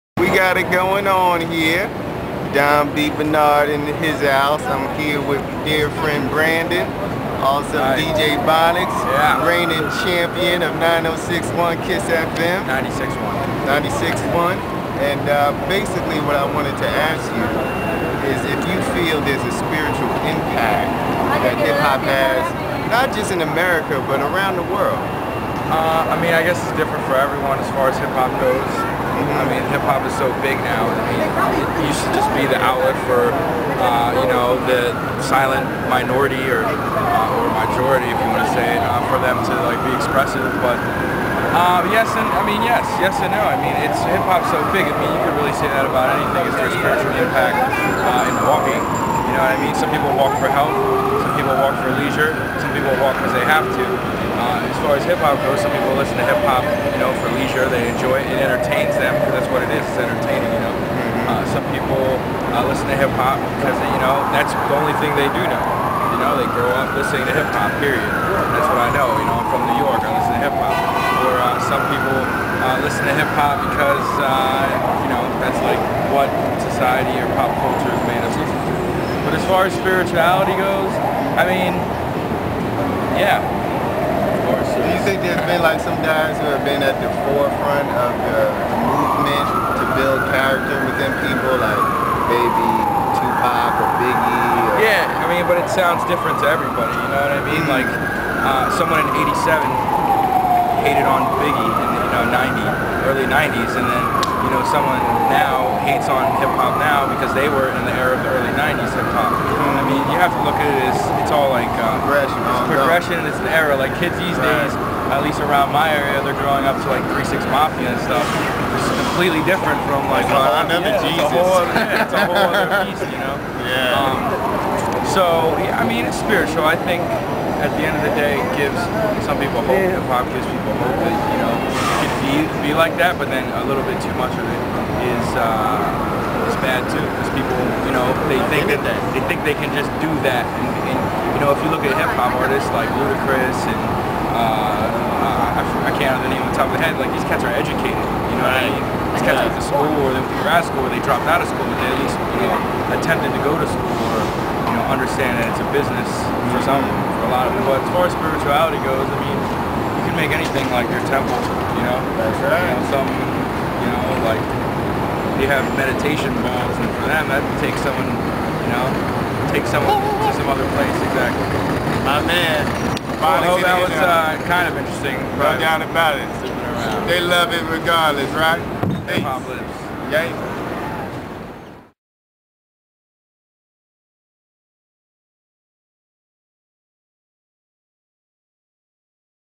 DJ Bonics on spirituality in Hip Hop
Don had the pleasure of meeting up with an old friend from the University of Pittsburgh, DJ Bonics, on the weekend of 777!  Here, they speak on the spiritual impact of Hip Hop.